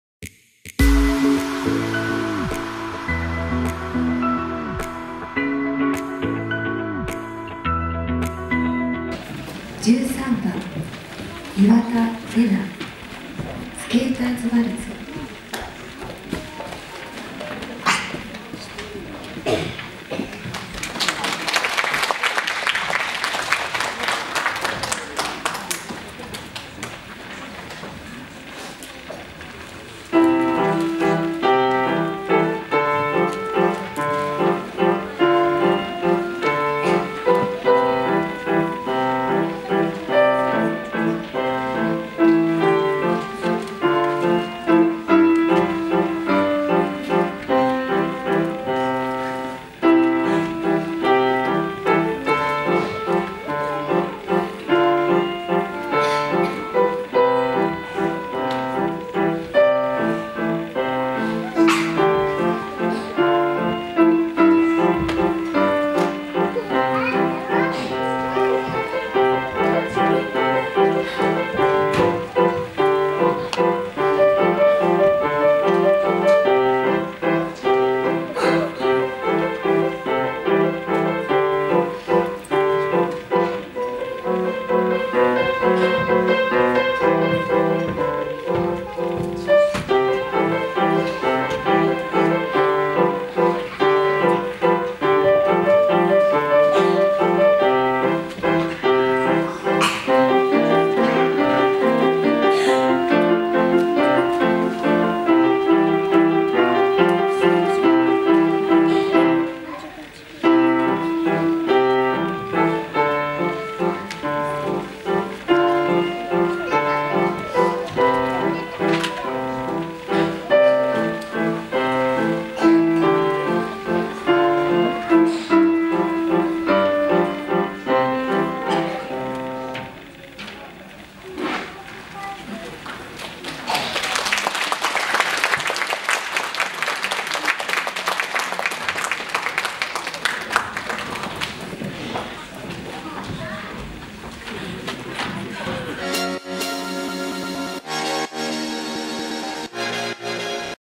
RENA PENTAS PIANO PERTAMA ll 5thun
Rena belajar piano dari umur 4 tahun 3 bulan dia pas pentas sudah 1 tahun belajar piano dia sangat senang skali belajar piano semoga selallu semnagt belajar piano.